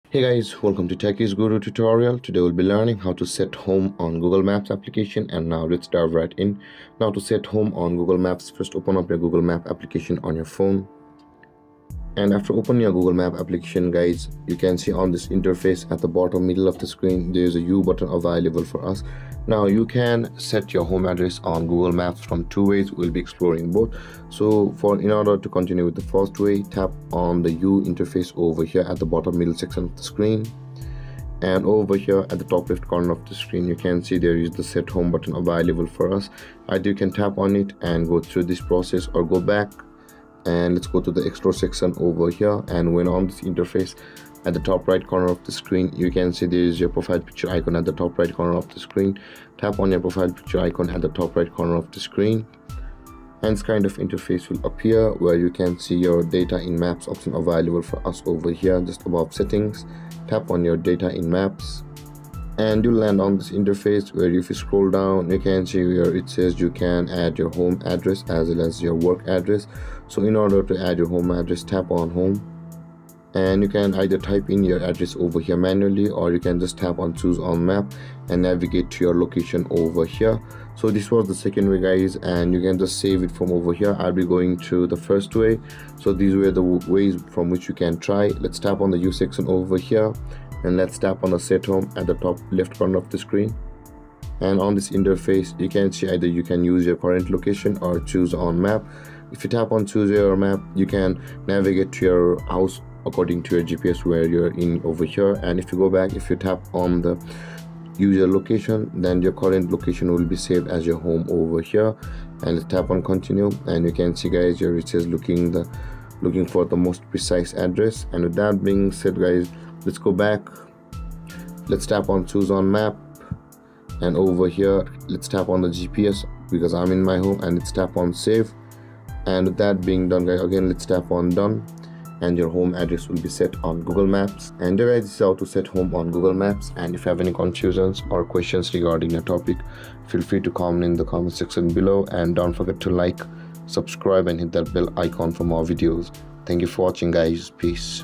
How to Set Home on Google Maps on Phone 2024?
🏠 How to Set Home on Google Maps on Phone 2024 | TechiesGuruTutorial

Need help setting your home address on Google Maps? In this simple tutorial, we’ll guide you through the steps to set your home location on Google Maps in 2024. Whether you've just moved or want to save time on navigation, we've got the steps you need!